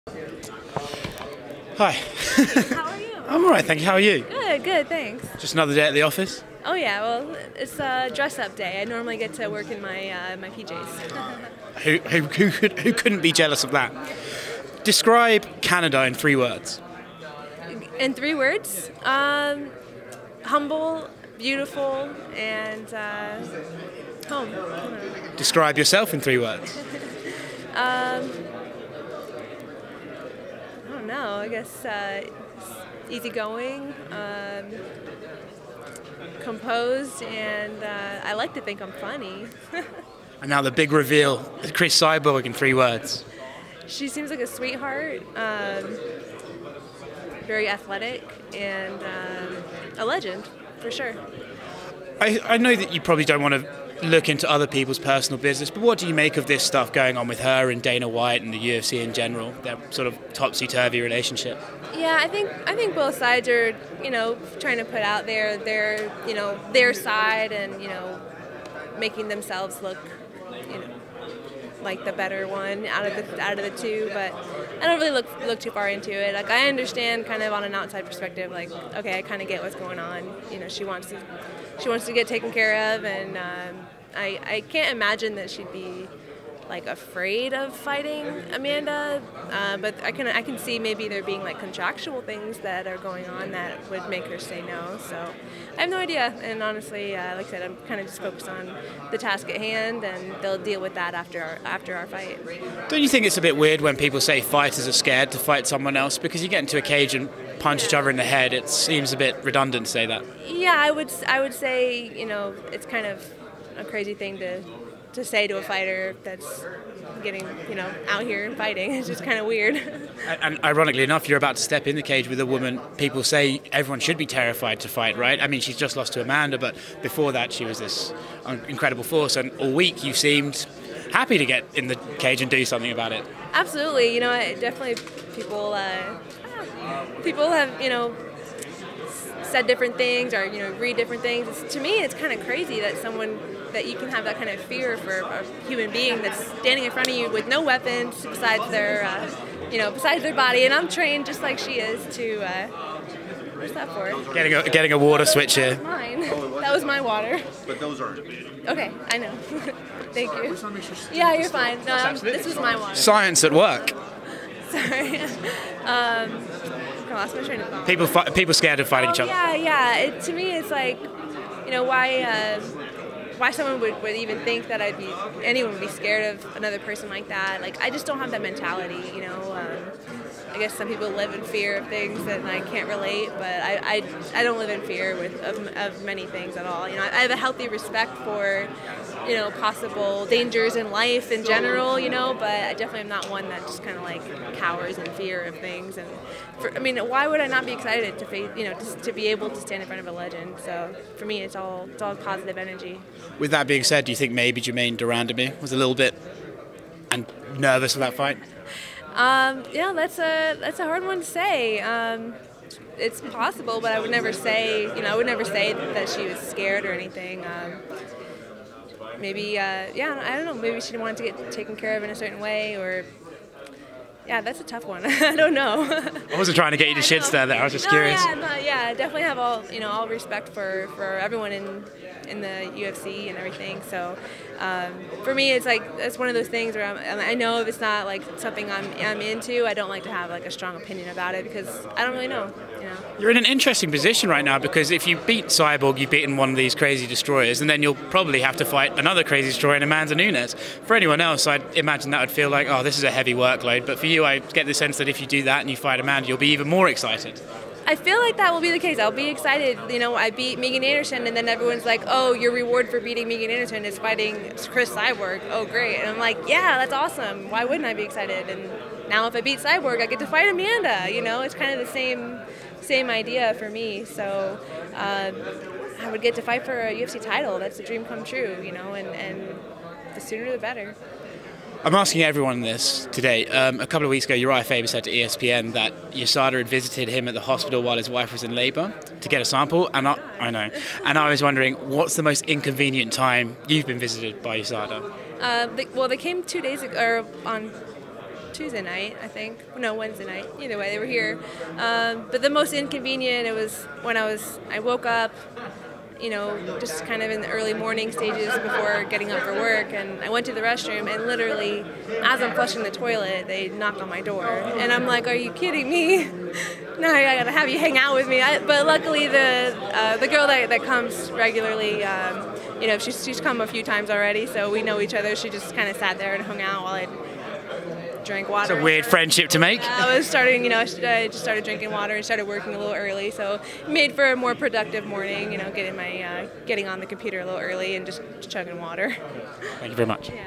Felicia Spencer excited by the prospect of fighting Cris Cyborg and Amanda Nunes back to back
Felicia Spencer spoke to Oscar Willis of TheMacLife at the UFC 240 Media Day in Edmonton, Canada.